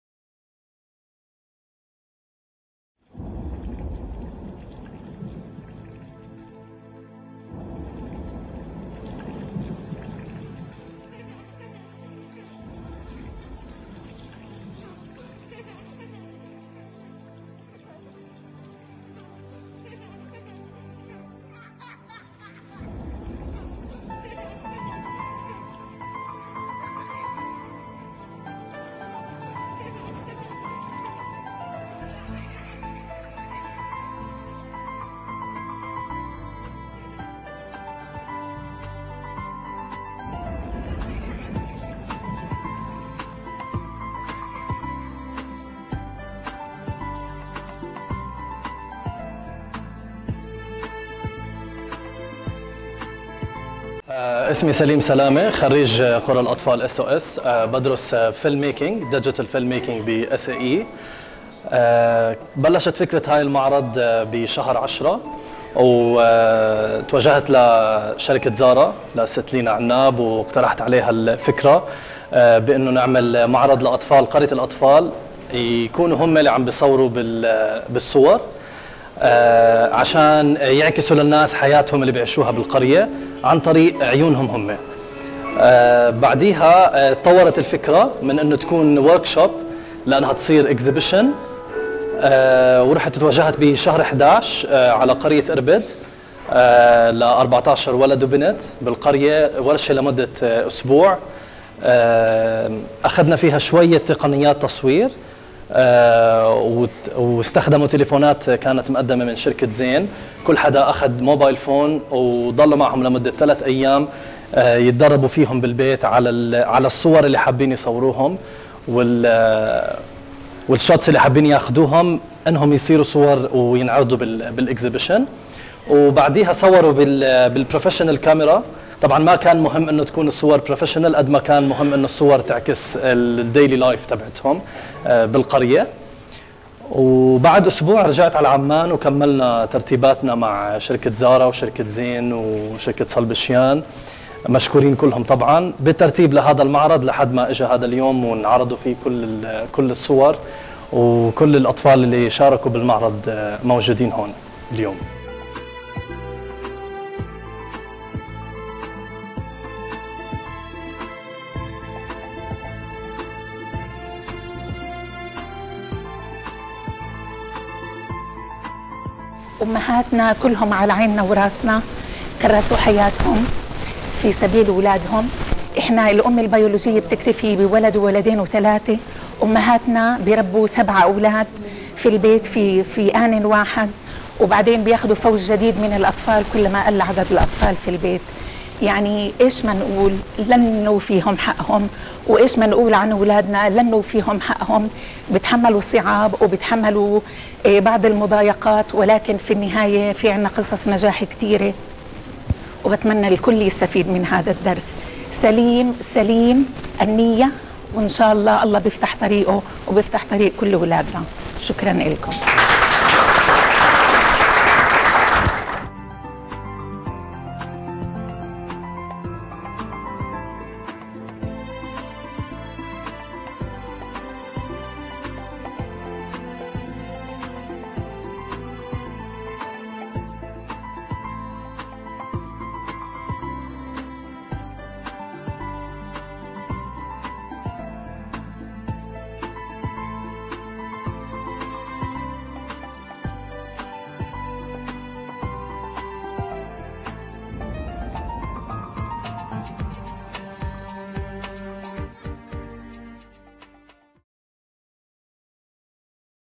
Home As We See It
For most children a normal life is having a place to call home, with parents who love you, take care of you and guide you through life’s obstacles. However, for some of the children who aren’t so lucky SOS Children’s Villages are able to provide stability and security along with love and care in a family setting. Children who have lost their parents or the support of their parents for one reason or another are looked after by SOS families that consist of a mother, brothers and sisters within the boundaries of a village, under the slogan “a loving home for every child”.

This exhibition is the output of participation of children at SOS Children’s Villages Jordan in self-expression and visual communication to others of their daily lives within their environment. SOS children may be less advantaged compared to other children living with their biological families but they are still loved, well looked after and have a good prospect for a successful life and a good stature in society.

"SOS children are nurtured and supported; they are far from forgotten"